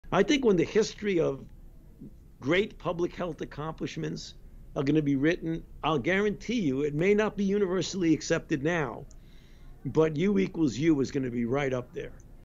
Dr. Fauci: U=U will be in history of great public health accomplishments -July 2022
"I think when the history of great public health accomplishments are going to be written, I guarantee you, it may not be universally accepted now,  but U=U is going to be right up there."

Courtesy of Karl Schmid and Plus Life July 2022